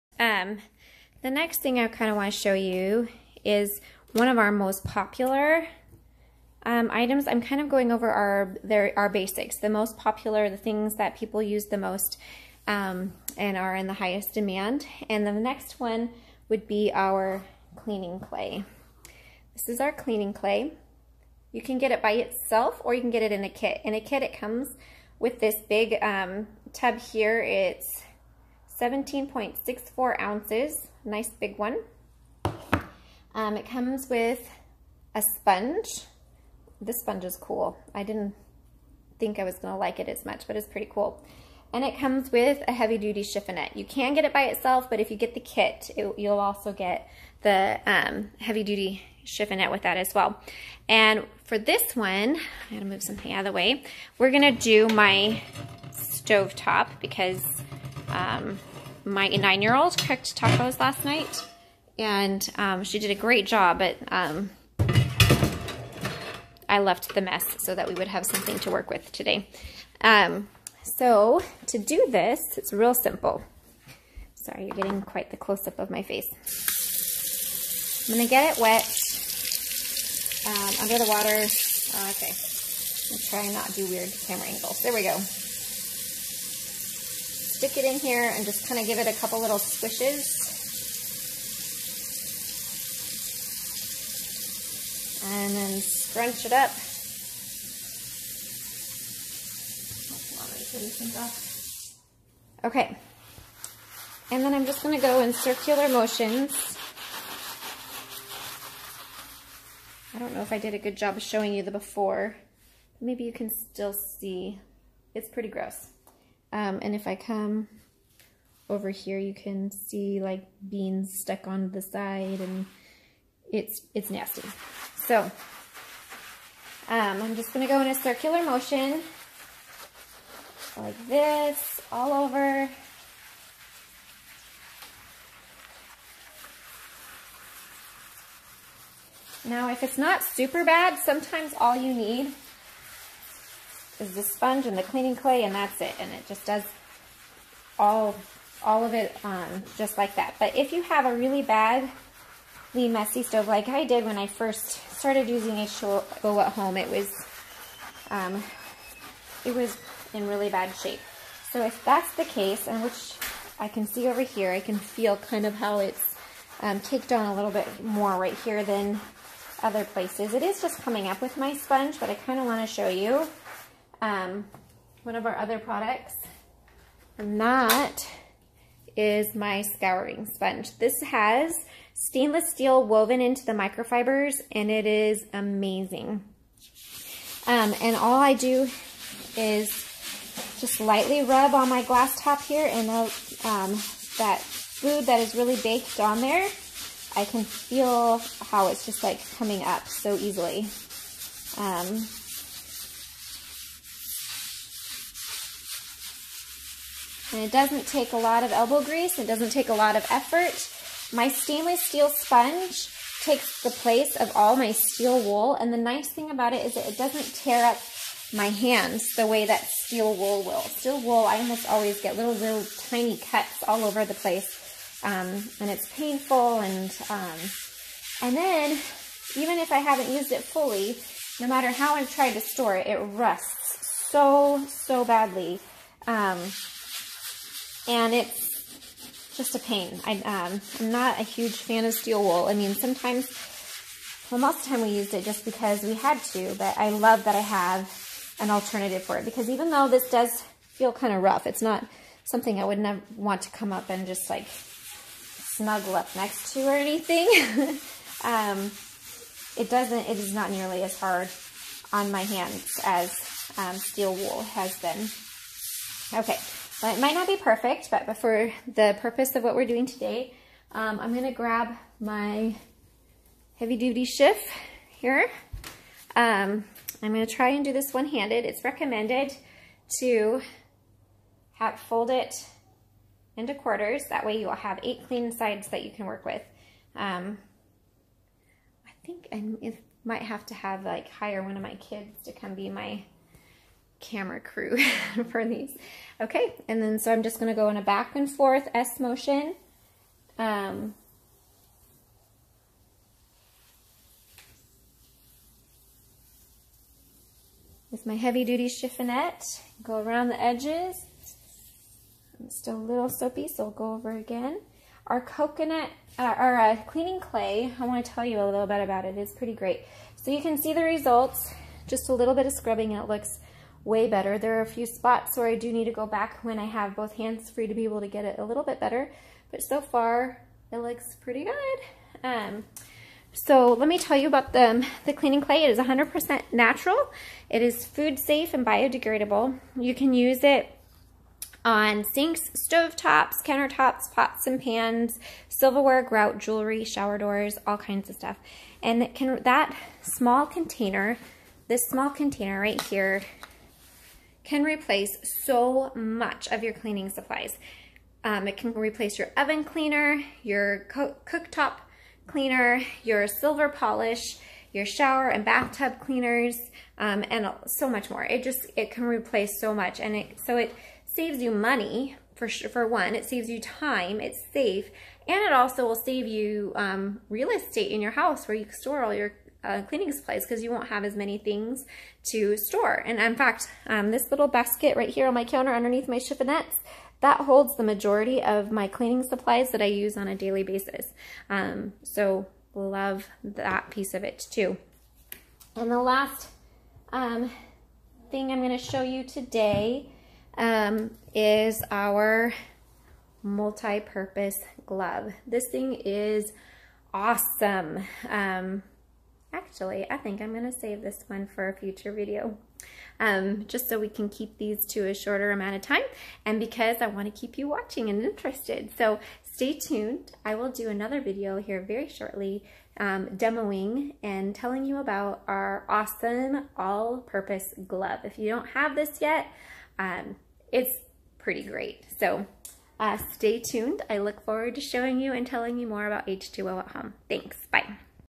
Heavy Duty Chiff, Cleaning Clay, and Scouring Sponge
This is a dynamite trio that I use on my really tough to clean hard surfaces.  The cleaning clay is one of our number one, best selling products, and for good reason.  This little jar will replace the bulk of your household cleaners, and it will last you a long time!